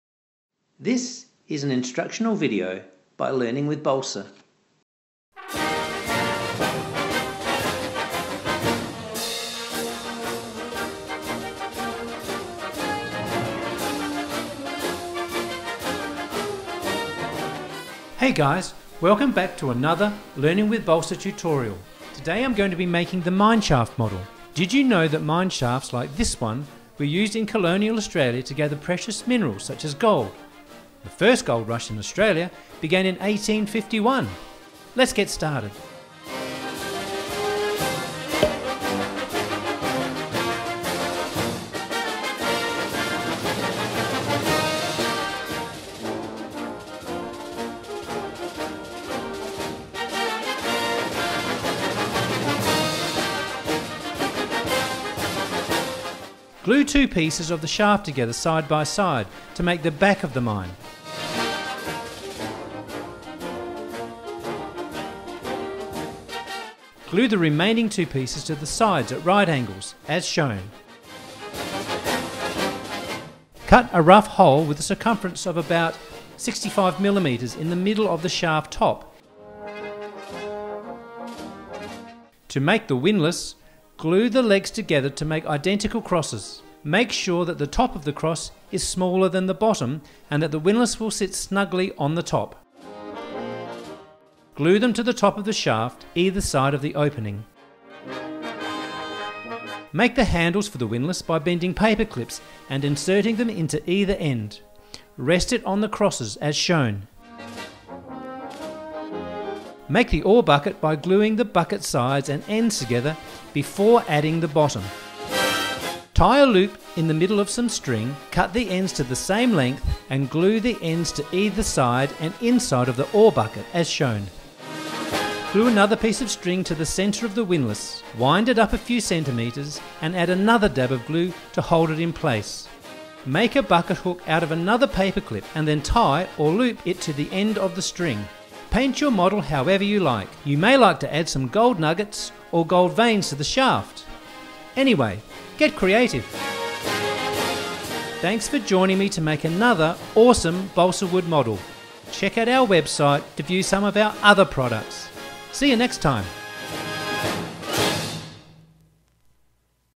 Mineshaft
A hands on learning, educational and instructional video on how to make a Mineshaft Model from Woodley at Learning with Balsa.